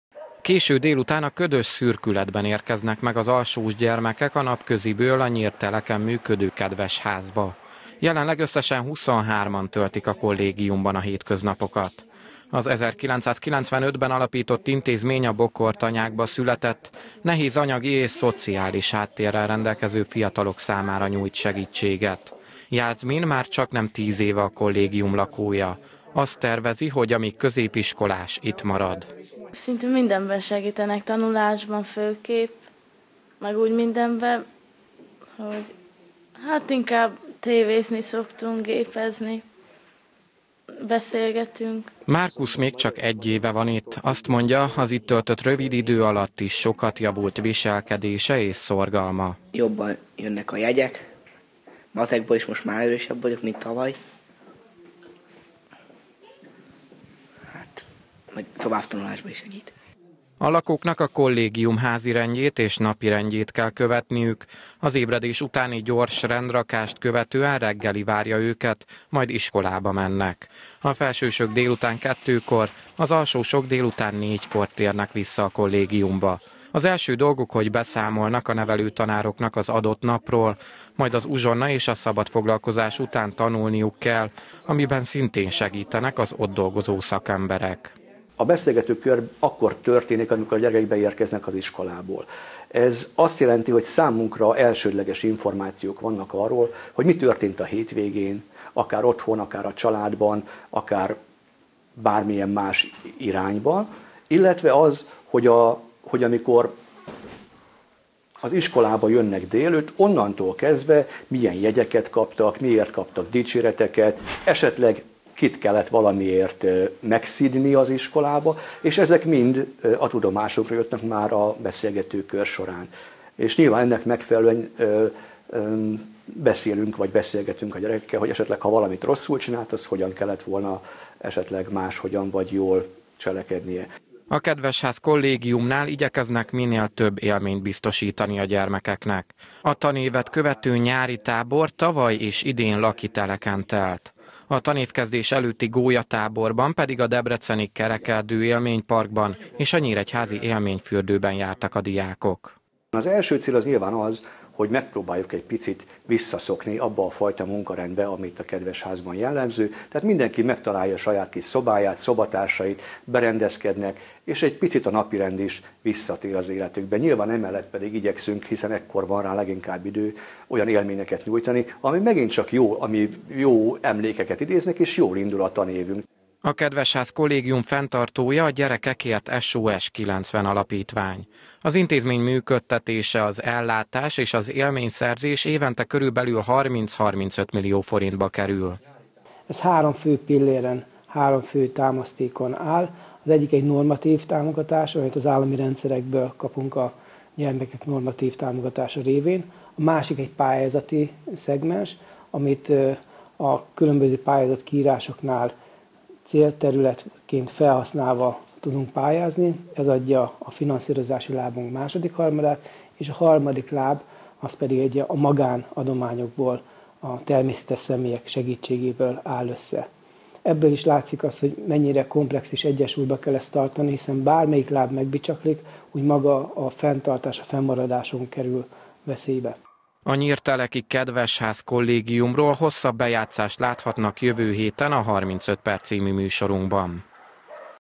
Kedvesház Nyírteleken
A tanyabokrokban élő hátrányos helyzetű gyerekeknek nyújt otthont, és tanulási feltételeket az iskolai sikerességhez a nyírteleki Kedvesház Kollégium. Az intézmény nem csak ellátást és szocializációs segítséget, hanem élményeket is biztosít a tanulók számára. A kollégiumnak jelenleg 23 lakója van, köztük olyan is akad, aki már csaknem 10 éve itt tölti a hétköznapokat.